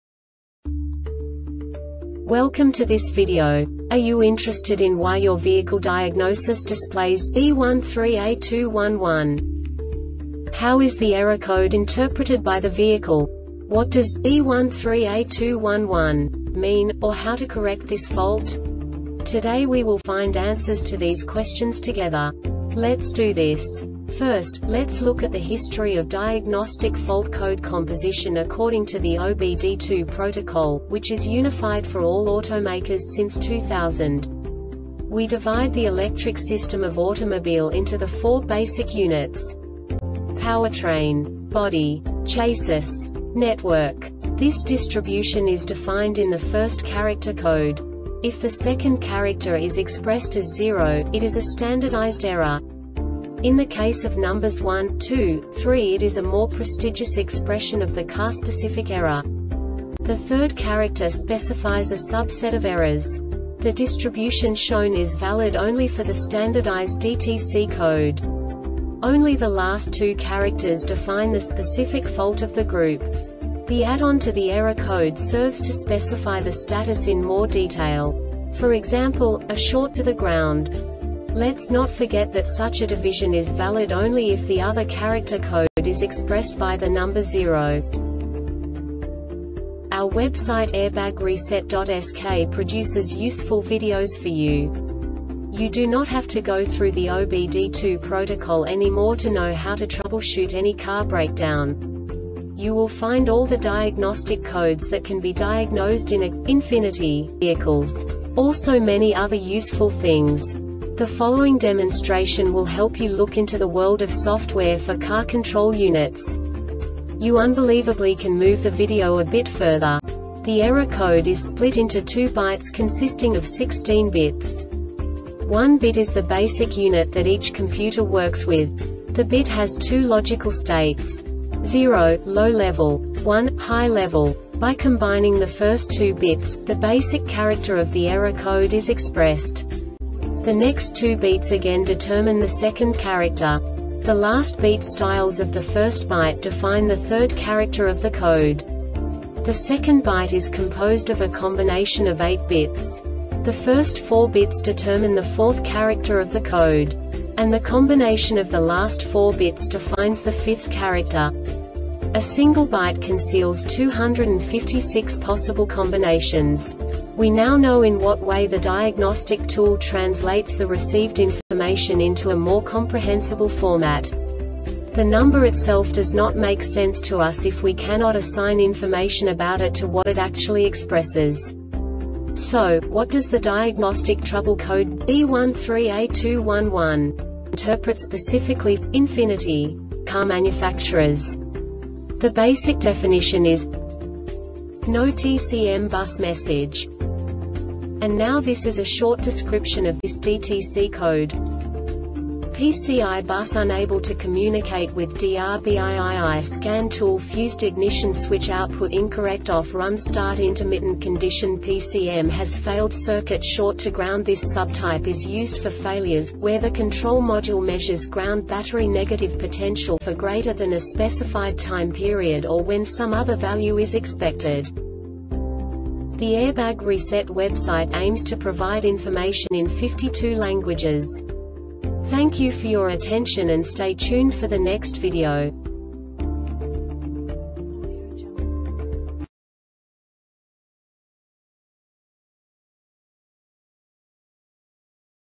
DTC Ford B13A2-11 Short Explanation
Contents:
0:21 Basic DTC analysis according to OBD2 protocol standard.
1:48 Insight into programming
2:58 A diagnostically understandable description of the Diagnostic Trouble Code
3:05 Basic error definition

Make:
Ford
Code:
B13A2 11
Definition:
No TCM Bus Message
Cause: